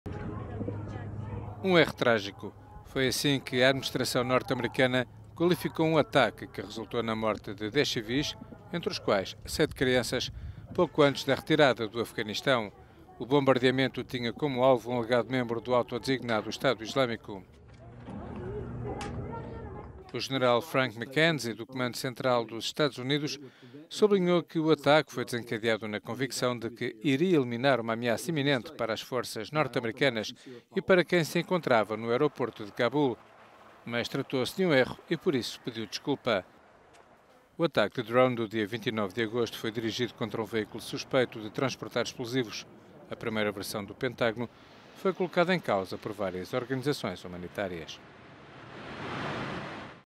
EUA pedem desculpa por "erro trágico"
O Pentágono reconheceu que o ataque contra um suspeito de pertencer ao autodesignado Estado Islâmico foi um erro e pediu desculpa. Bombardeamento matou 10 civis, 7 dos quais crianças.